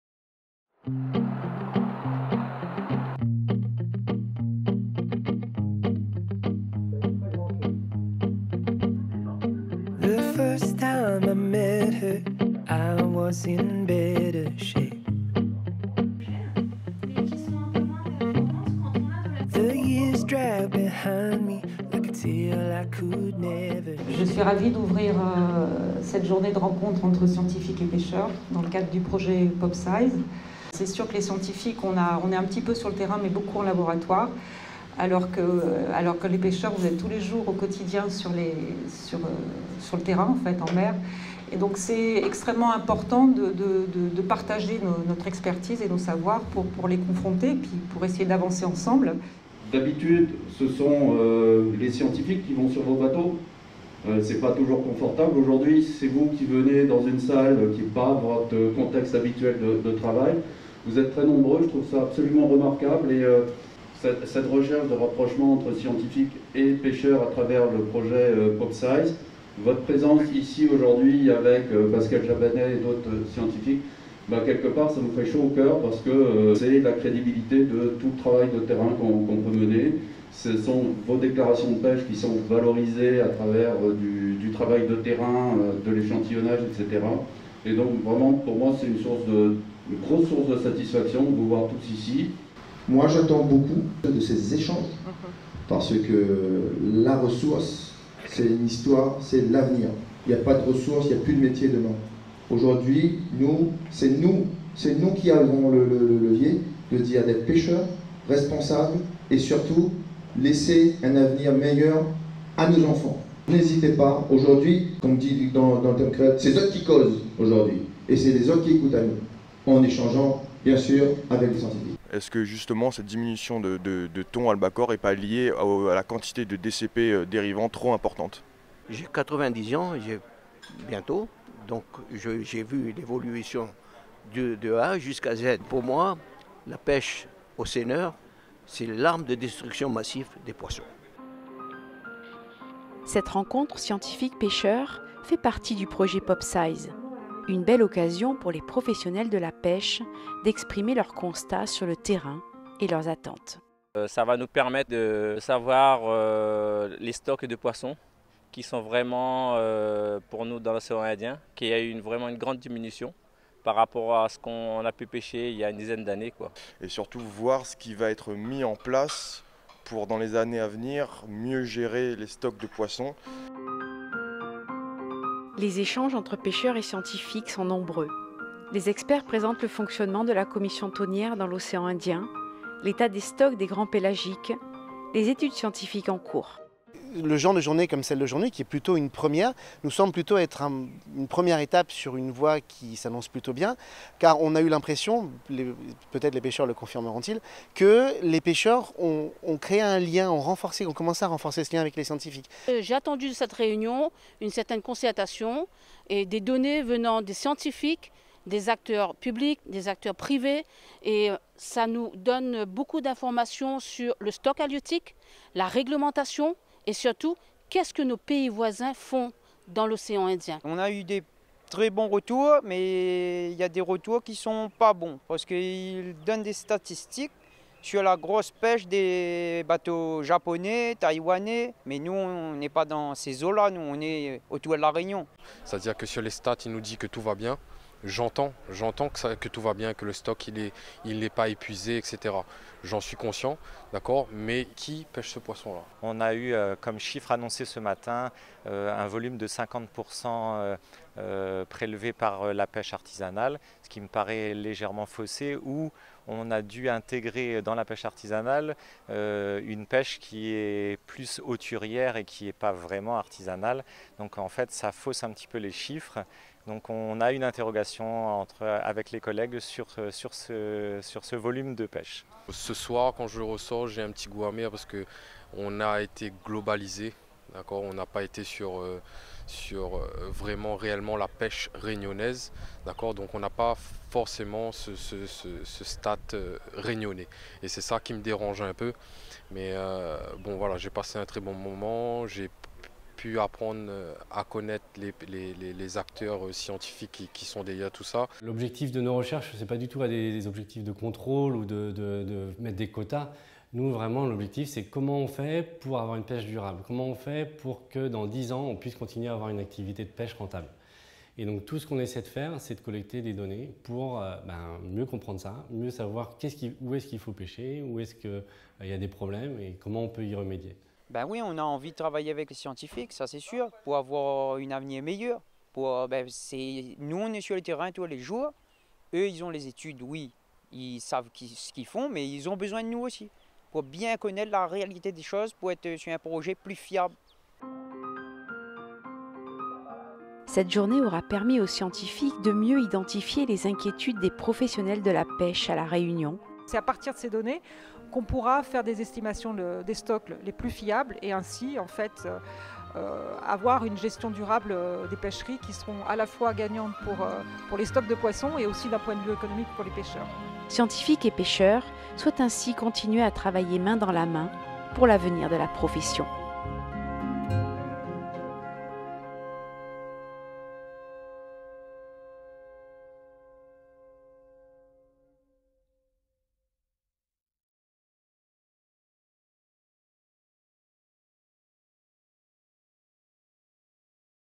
La mer dans tous ses états : rencontres pêcheurs-scientifiques sur la pêche réunionnaise
Rassemblés pour une 1re rencontre dans le cadre du projet POP-SIZE, pêcheurs et scientifiques spécialistes des pêches ont échangé sur les solutions pour assurer une pêche durable à la Réunion. Les professionnels de la pêche ont pu valoriser leur connaissance précise du terrain et exprimer leurs attentes et leurs inquiétudes face à la durabilité des stocks de poissons. Les chercheurs ont quant à eux présenté les derniers résultats scientifiques et les dernières données sur la pêche autour de l’île. Le but de la rencontre était d’amorcer une concertation pour mettre en place une gestion durable des pêcheries, profitables à la fois pour les stocks de poissons et pour les ressources économiques des pêcheurs.
Pour en savoir plus sur le projet POP-SIZE qui vise à estimer la taille des populations de poissons grâce à la génétique
Réalisation : La Cerise sur le Gateau Prod
Avec la participation de : Pascale Chabanet, Estelle Crochelet, Chrystelle Delord, Alain Fonteneau, Nicolas Hibon, Sandra Hohmann, Francis Marsac, Natacha Nicolic.
Série: La mer dans tous ses états : épisode 1
© La Cerise sur le Gâteau Prod – IRD 2021